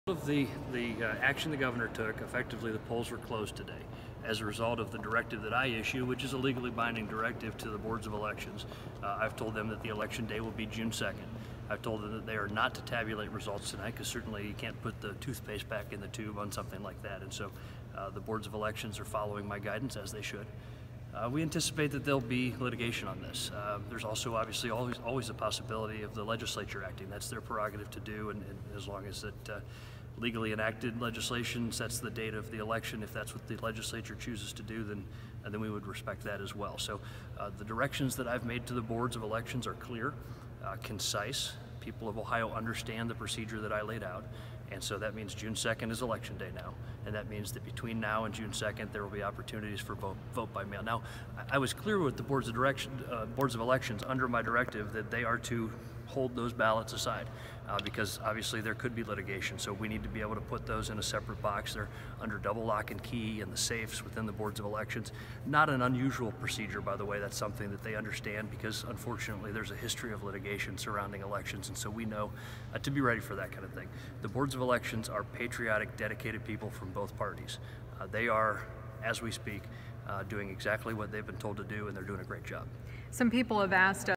LaRose clarifies election actions
I had an impromptu news conference in the state house rotundra, Ohio secretary of state Frank LaRose discusses changes to the election and the securing of ballots